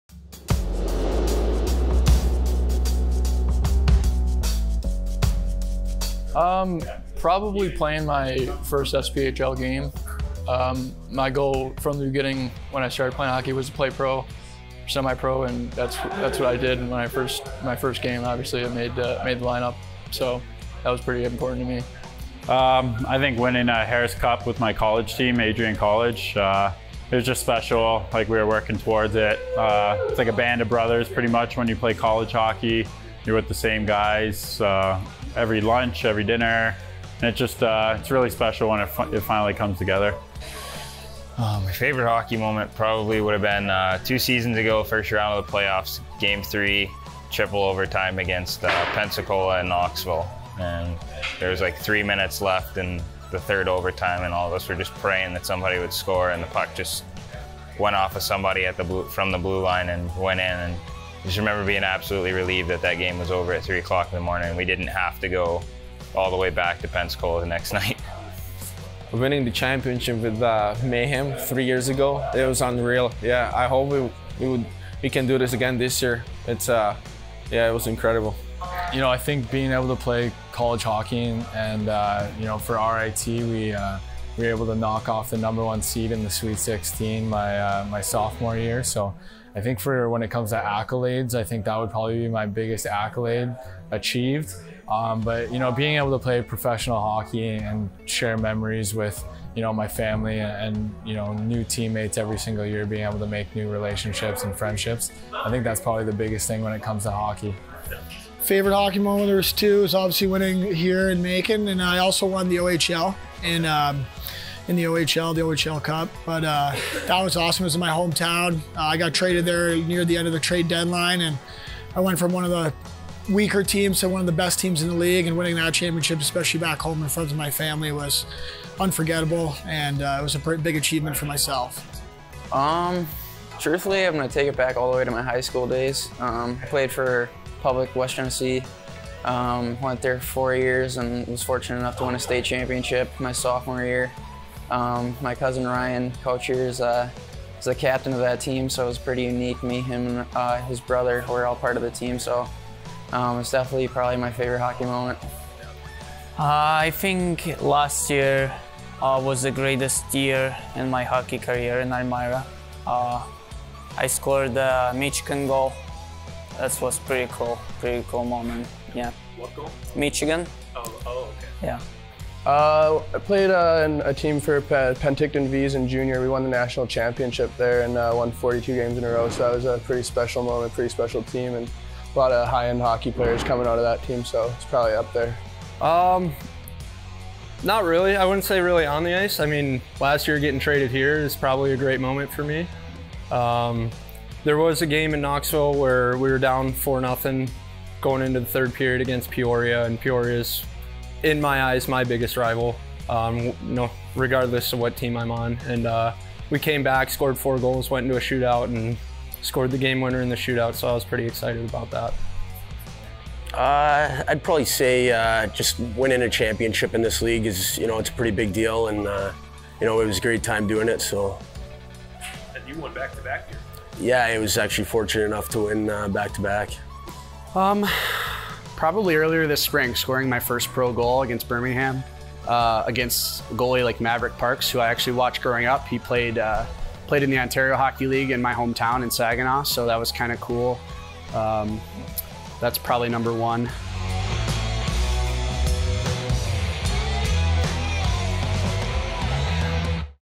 Meet the Mayhem Episode 3 | Favorite Hockey Memory
In our third installment of "Meet the Mayhem," we asked the players to share their favorite hockey memories from their careers!

Editing by Eric Robbins Media